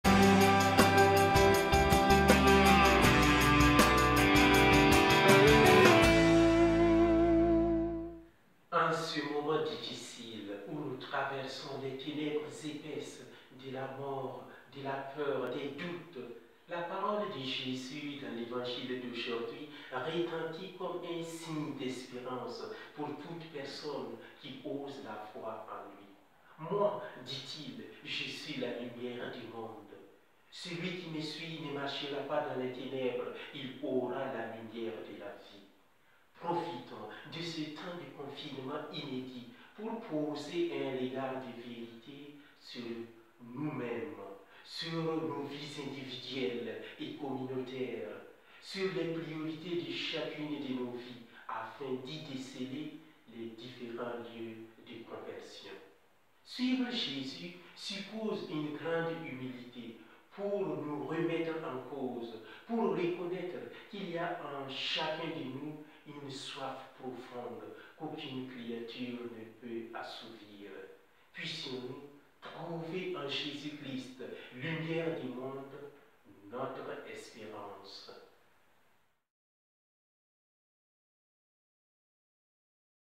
Confinésconnectés n°16
[Confinésconnectés n°16] Trouver du temps, aujourd’hui, pour contempler Ta lumière en moi… - avec le père Bertrand Sawadogo de la paroisse Saint Jean Baptiste de Belfort.
« Celui qui me suit ne marchera pas dans les ténèbres, il aura la lumière de la vie. » (Jn 8, 12-20)